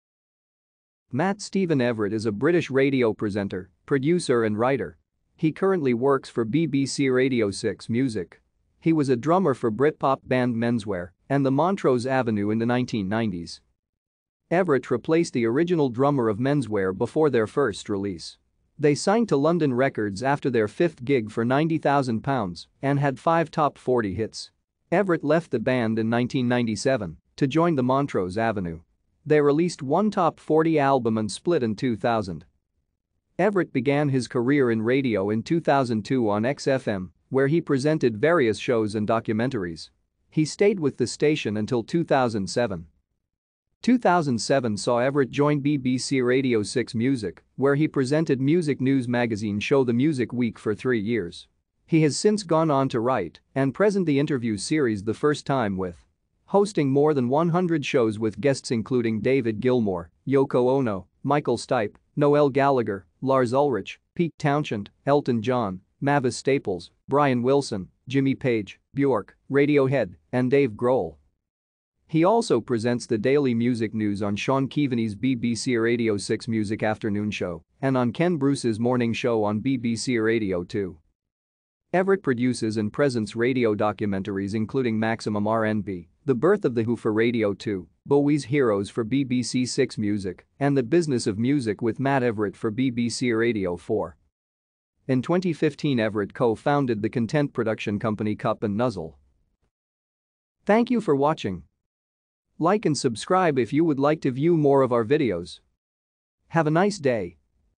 Matt Everitt
Matt Stephen Everitt  is a British radio presenter, producer and writer. He currently works for BBC Radio 6 Music. He was a drummer for Britpop band Menswear and The Montrose Avenue in the 1990s.